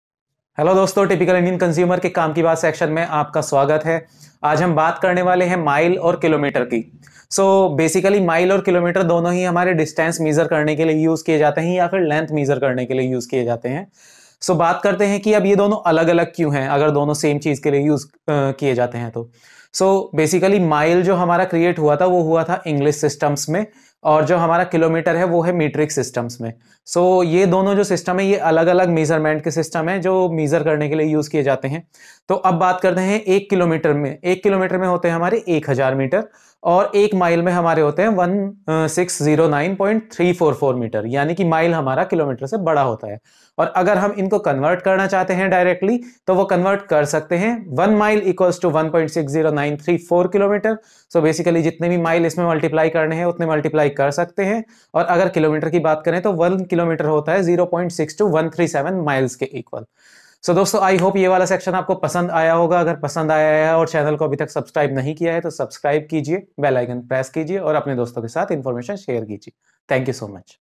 Mile vs kilometer shortest way to convert...
What is mile and kilometer? why they are used? How to convert them?
Shortest video check out.